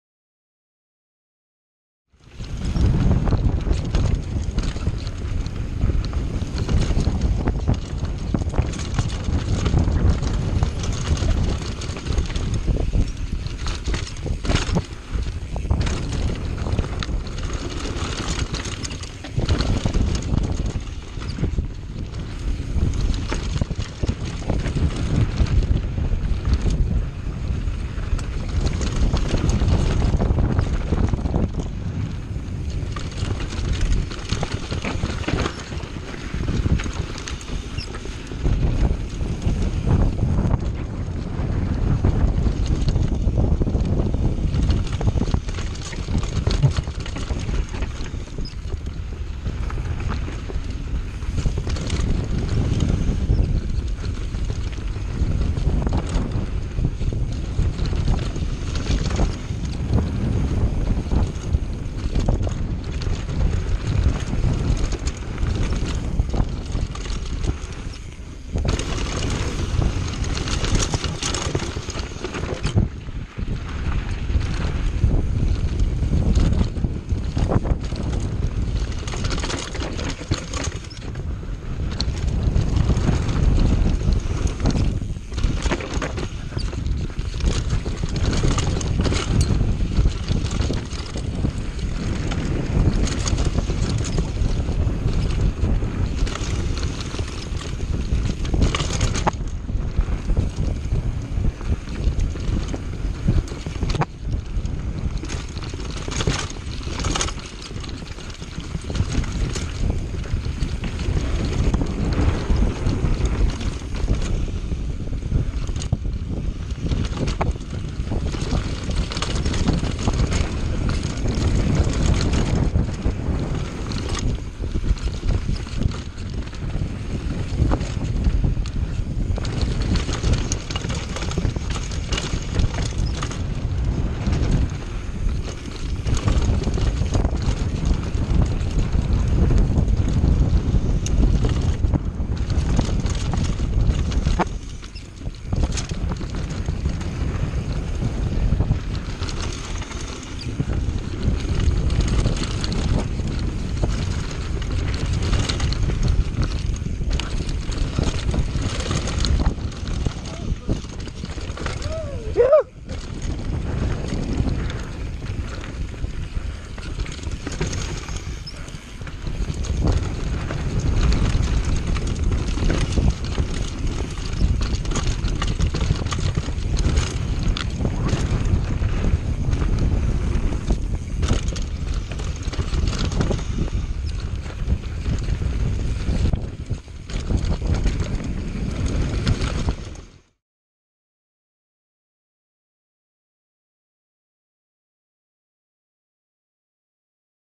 Mountain Biking Mt. Bachelor in Bend
Three minutes of mountain biking the Rattlesnake trail at Mt. Bachelor in Bend, Oregon.